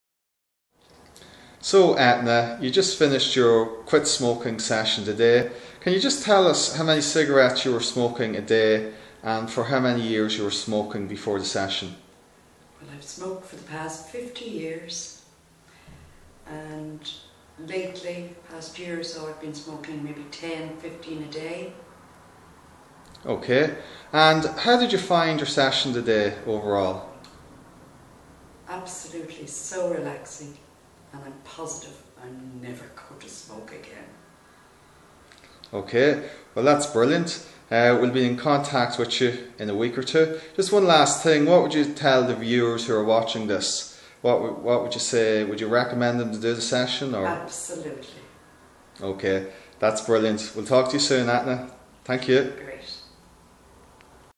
Stop smoking hypnosis Dublin
Stop smoking hypnosis in one session client video testimonial for more video and information. Advanced Hypnotherapy Dublin